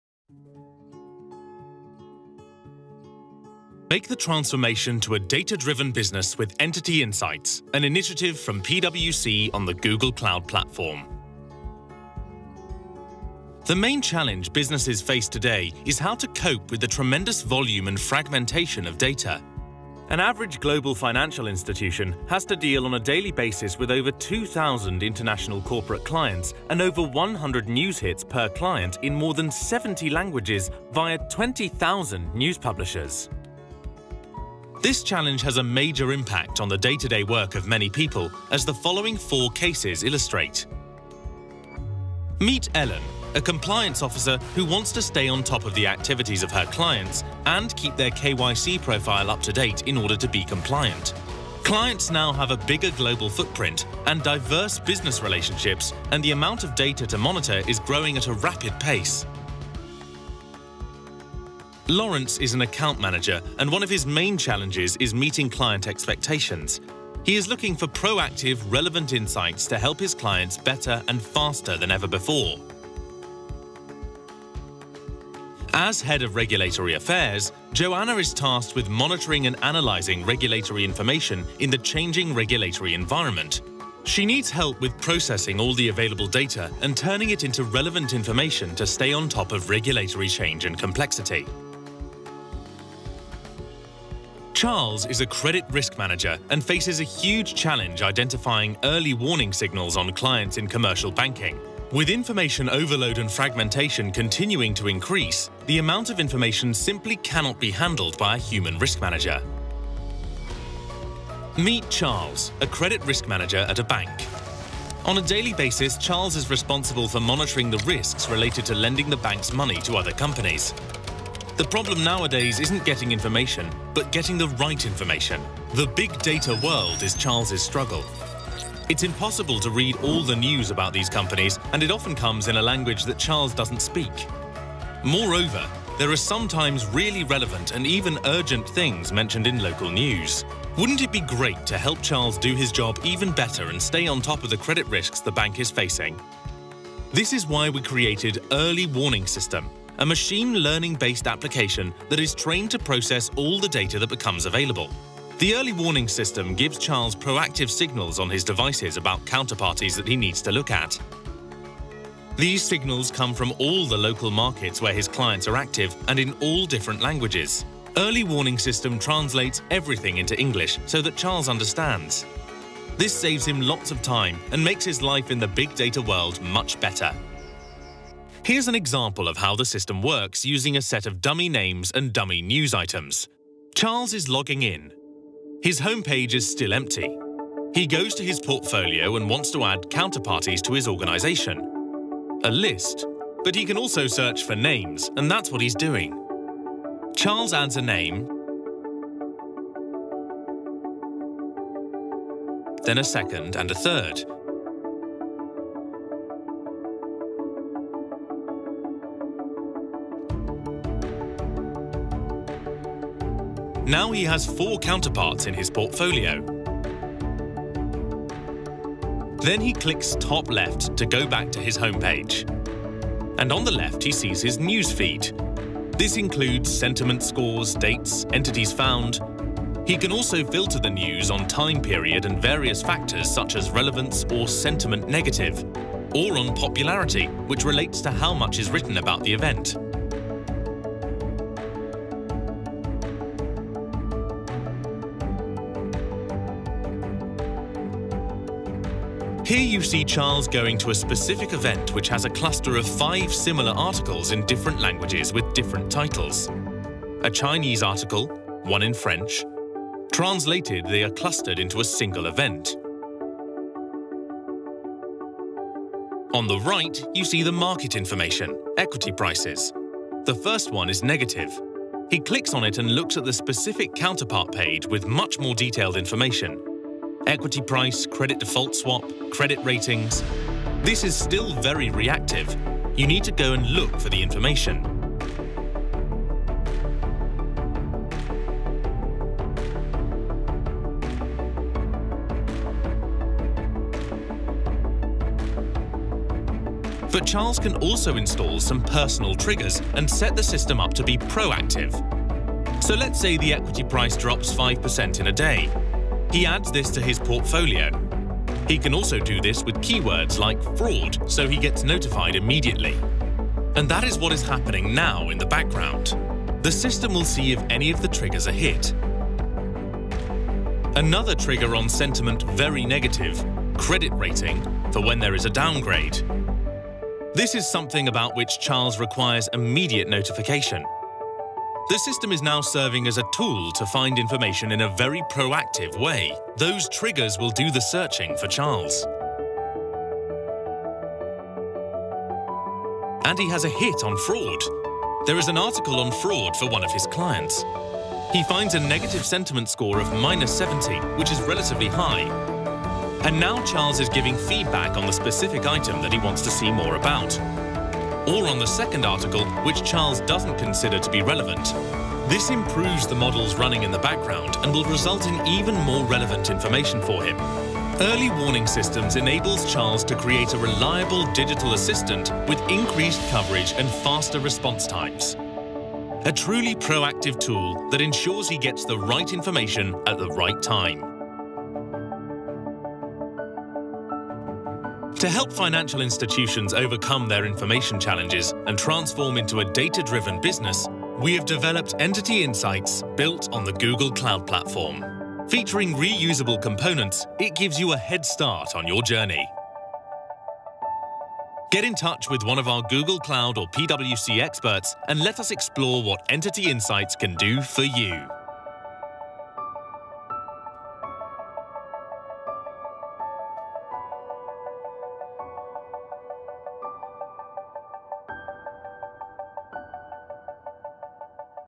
PwC - Entity Insights: Early Warning System
Organisaties hebben te maken met gegevens van vaak duizenden (internationale) klanten. De uitdaging is om greep te houden op deze overvloed aan, vaak sterk gefragmenteerde, informatie. Om organisaties hierbij te helpen hebben we Entity Insights ontwikkeld. Voorbeeld uitdaging met Early Warning System (vroegtijdig waarschuwingssysteem): Charles is een kredietrisico-manager die voor grote uitdagingen staat op het vlak van vroegtijdige waarschuwingssignalen over de tegenpartijen in de commerciële banksector. Zijn probleem is het vinden van de juiste informatie te midden van een hele berg gegevens, waarvan een groot deel wordt gepubliceerd in talen die Charles niet spreekt. Daarnaast is het belangrijk dat hij relevante berichten kan filteren uit een grote hoeveelheid nieuwsberichten. Lees meer op